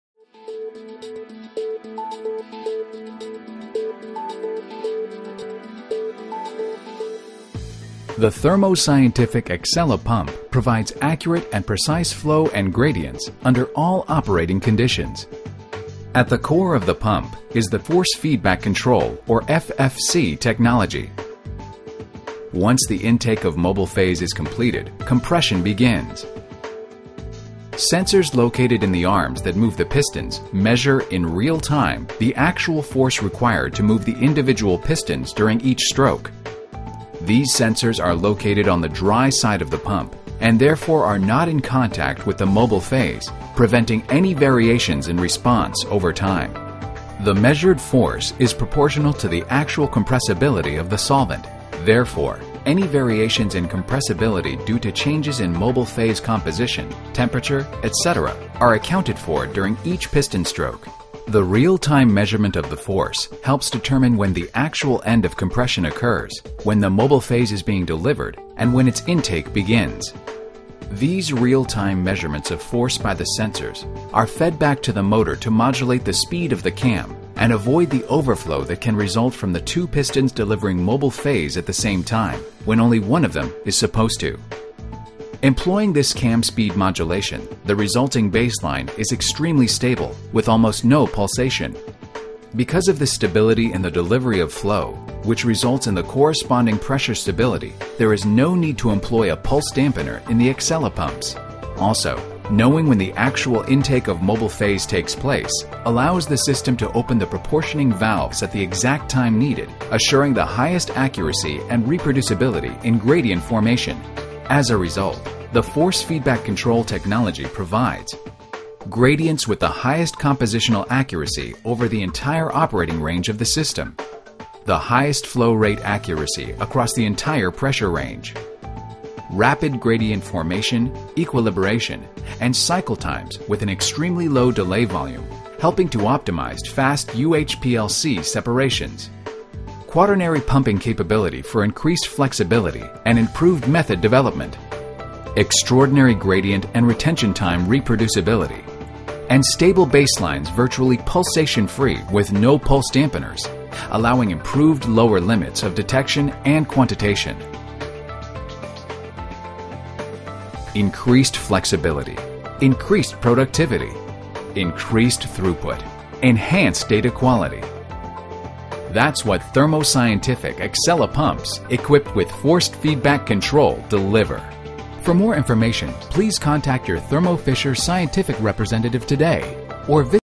Thermo Fisher Scientific Accela HPLC Force Feedback Control (FFC)
A comprehensive look on Thermo Scientific Accela's new FFC